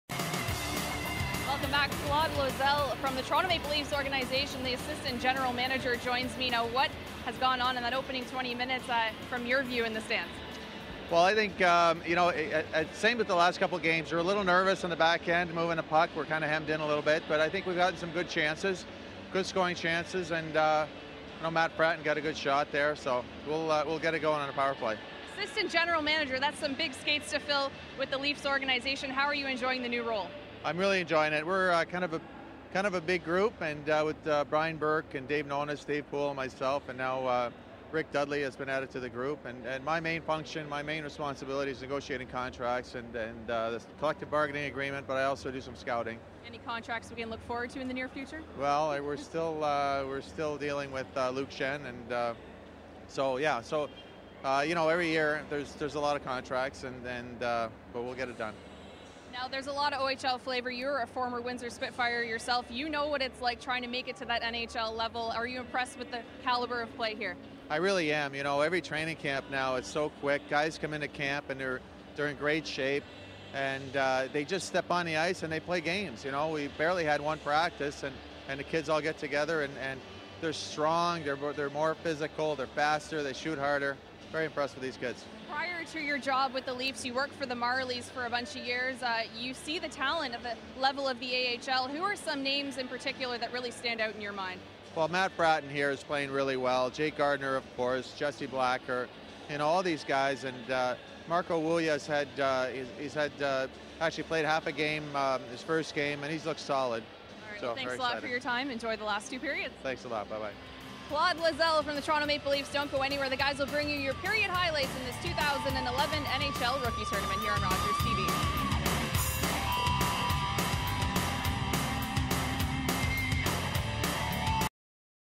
Claude Loiselle- Assistant General Manager of the Toronto Maple Leafs
Michelle Farrugia interviews Claude Loiselle, Assistant General Manager of the Toronto Maple Leafs at the 2011 NHL Rookie Tournament.